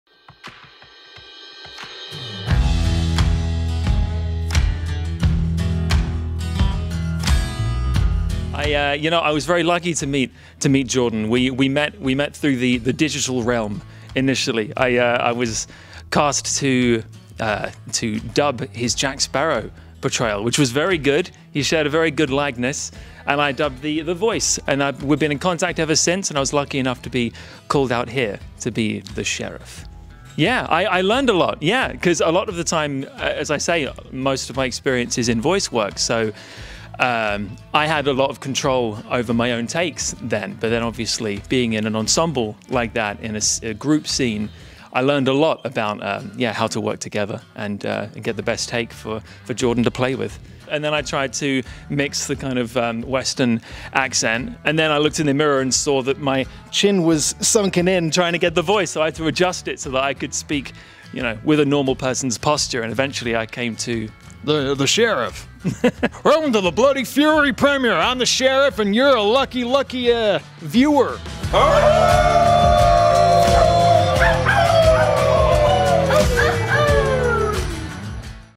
INTERVIEW BLOODY FURY : STEVE GOODY
Pour la sortie de film Bloody Fury rencontre avec Steve Goody lors de la Première du film au cinéma Mac Mahon à Paris.
For the release of the film Bloody Fury meeting with Steve Goody during the Premiere of the film at the Mac Mahon cinema in Paris.